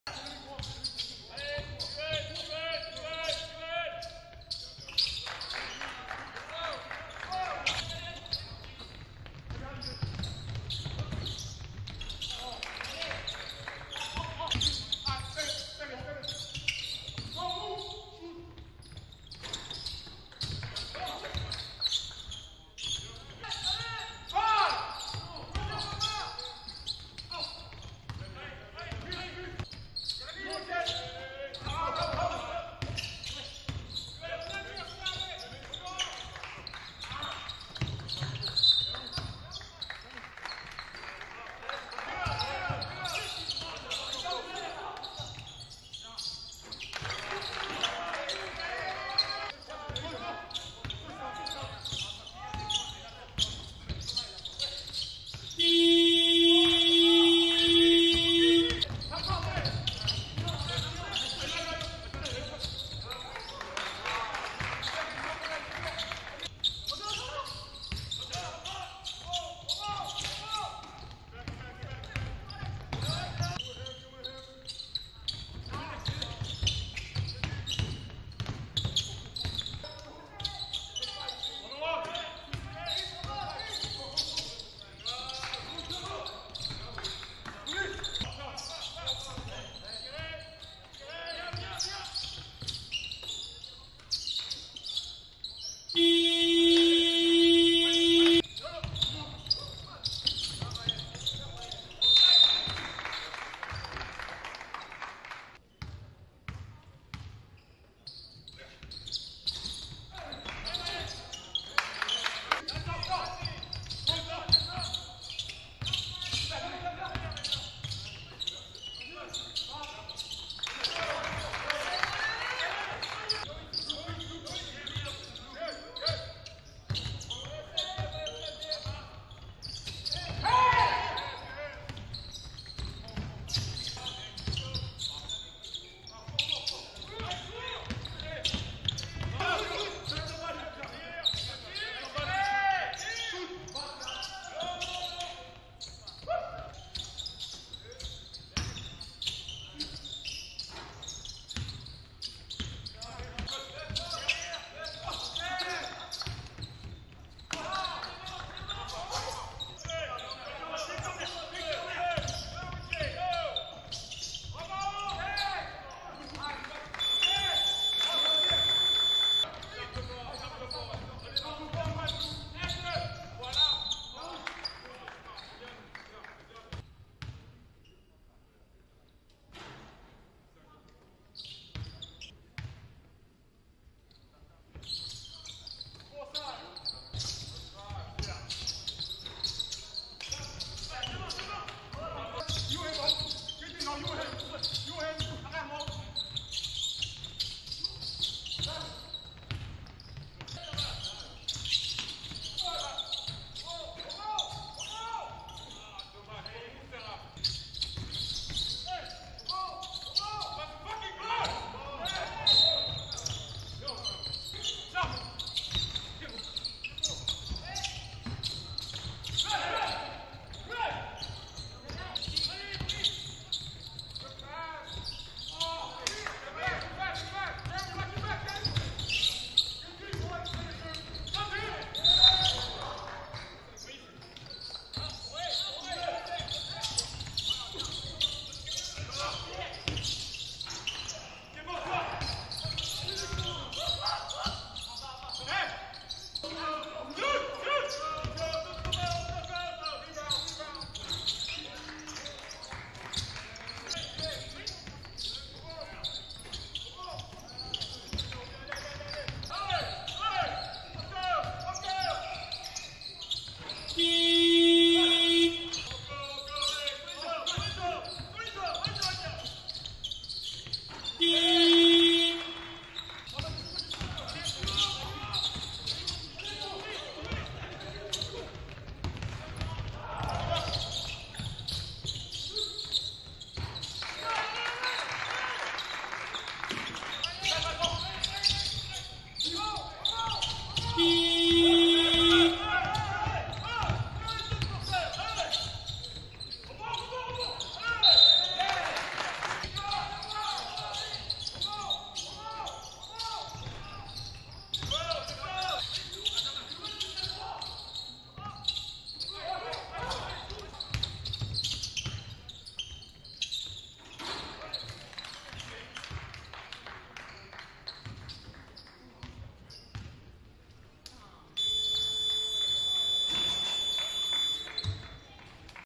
Ousmane Dieng vs Caen Basket Calvados 2.2.21 | 20pts (7-18FG, 4-93pt, 2-3FT), 14Reb, 2Ast, 2Stl, 4TO
Every shot, assist, rebound, steal, block, & turnover from Centre Federal Du Basket-ball wing Ousmane Dieng's 2-2-21 game against Caen Basket Calvados. Get the FULL picture, not just the highlights.